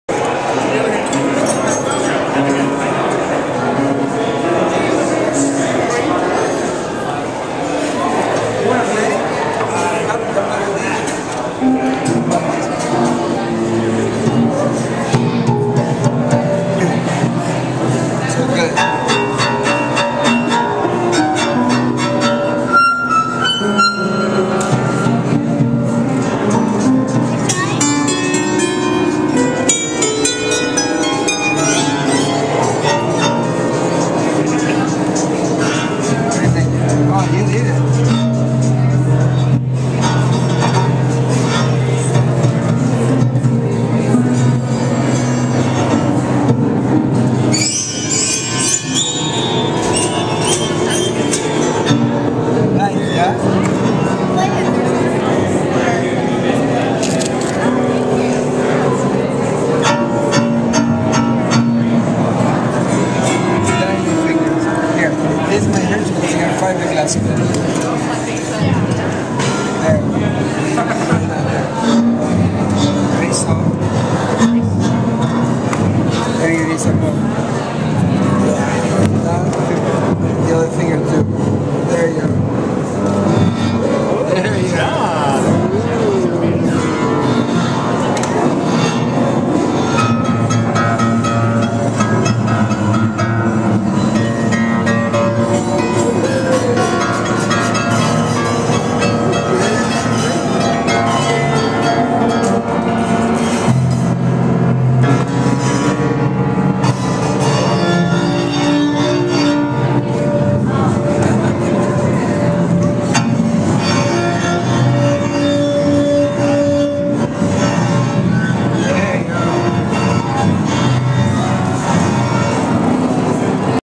ISKME's Design Lab at Maker Faire
Maker Faire attendees explore Mauro ffortisimo's piano liberado (deconstructed piano) and prototype their design ideas for the school of the future in ISKME's Design Lab,